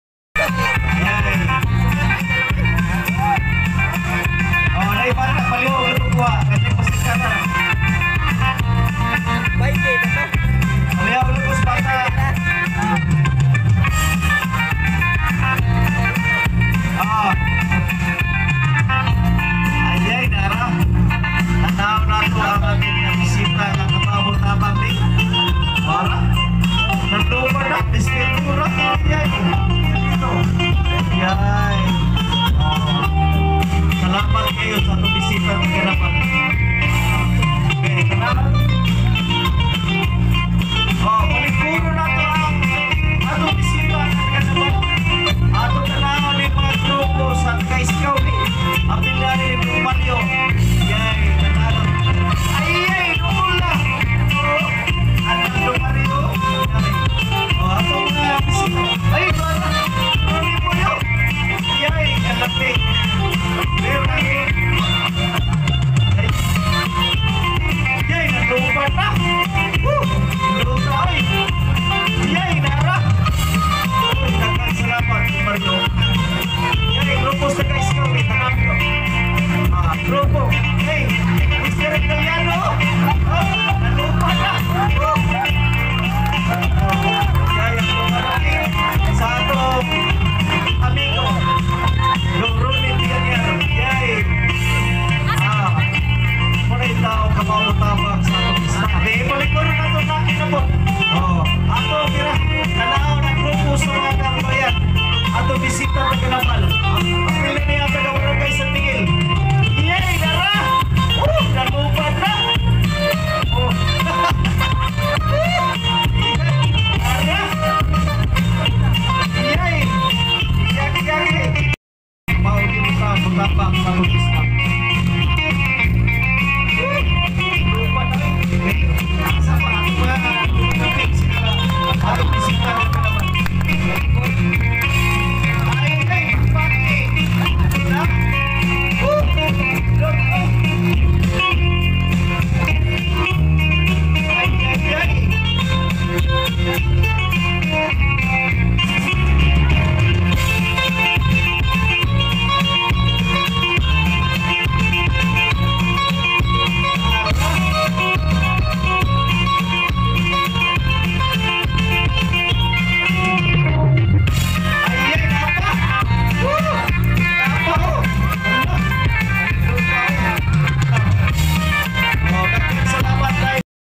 Kuratsa viral shorts viralvideo viralshorts kuratsa philippines
Kuratsa is one of the traditional dances in Leyte and Samar province. It is widely danced especially during fiesta, family reunions, and weddings. It is referred to as a courtship dance where dancers imitate courtship movements of rooster and a hen in a creative way.